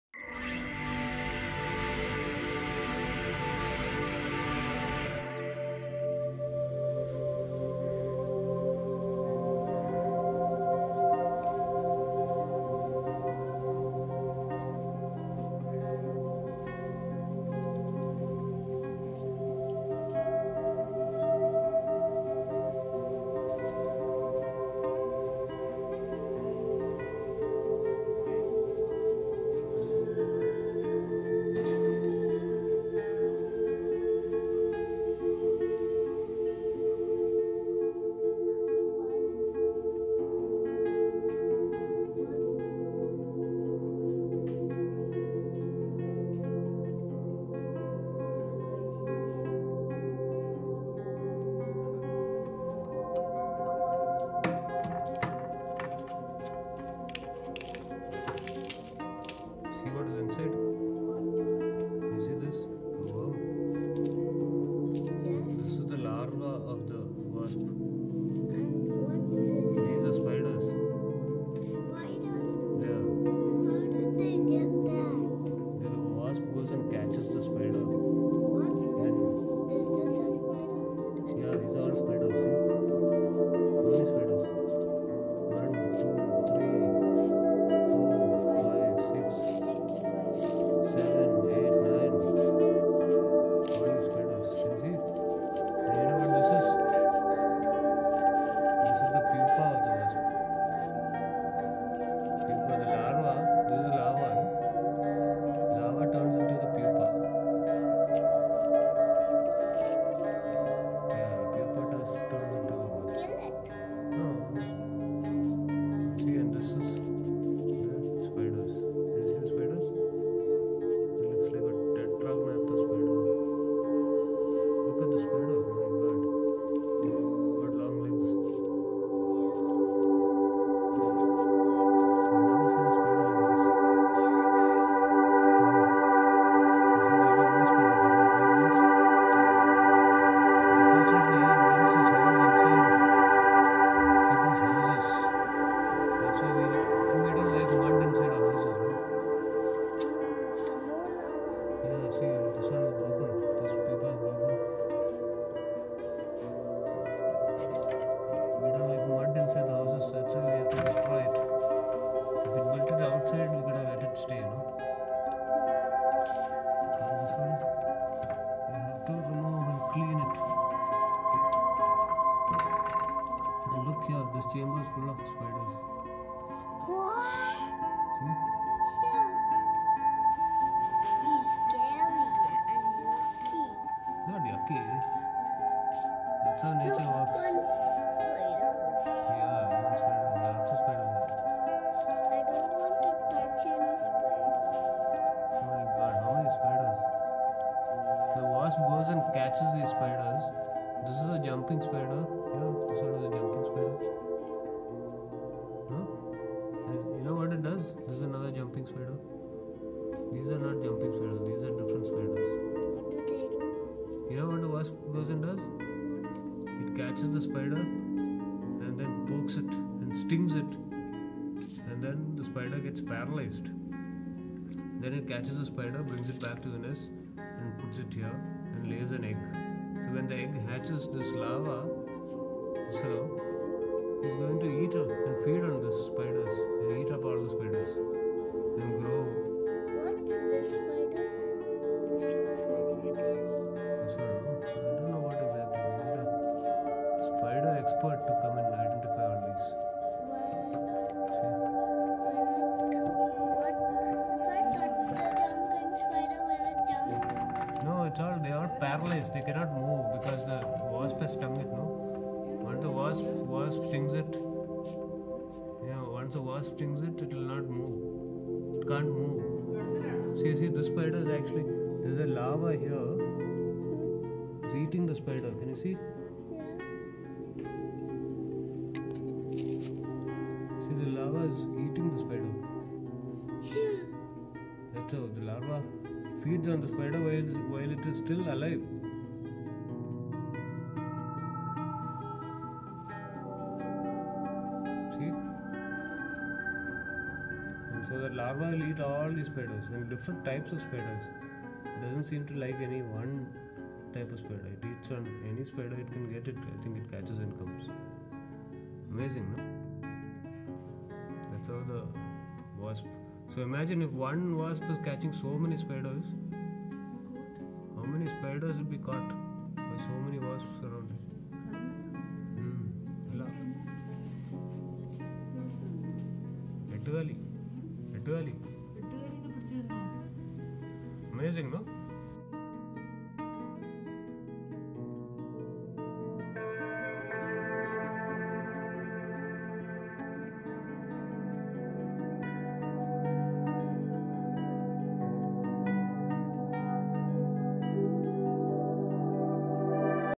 Mud wasp catches over 25 spiders for her nest!
Mud-dauber wasps catch, sting and paralyze spiders, which they then take back to their nest. The spiders are stocked up in the nest along with their eggs. When the eggs hatch, the larva will feed on the still alive but paralyzed spiders. Here I open the nest of one such wasp to find the spiders. THIS NEST WAS BUILT INSIDE THE BEDROOM. Some of us actually dont want insects and cobwebs taking over the bedroom. Kindly stop posting comments about what a crime it was to destroy a wasp nest for a video while living in your own sanitised environment. This was not done purely for the video. We live in a rural area where wasp nests are a nuisance and we periodically clean out the wasp nests. I just happened to be there that day and filmed was asked to help clean the nest as it was built on the latch of the door and we couldnt use the latch. If we do not clean it, the whole house would be full of patches of mud.